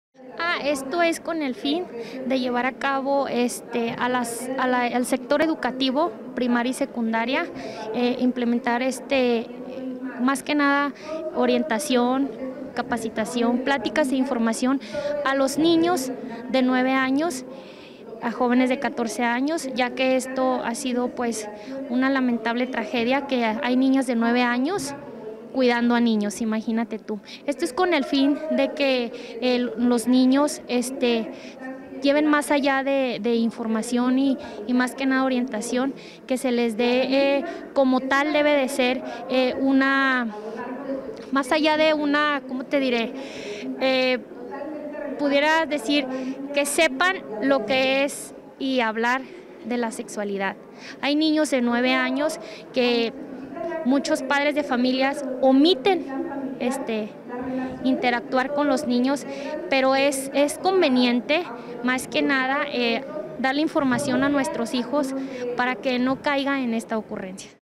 Diputada de Morena propone implementar educación sexual en escuelas
Es con el fin de implementar al sector educativo, primaria y secundaria orientación, pláticas, capacitación e información en este tema; señaló Ofelia Rentería, diputada local de Morena.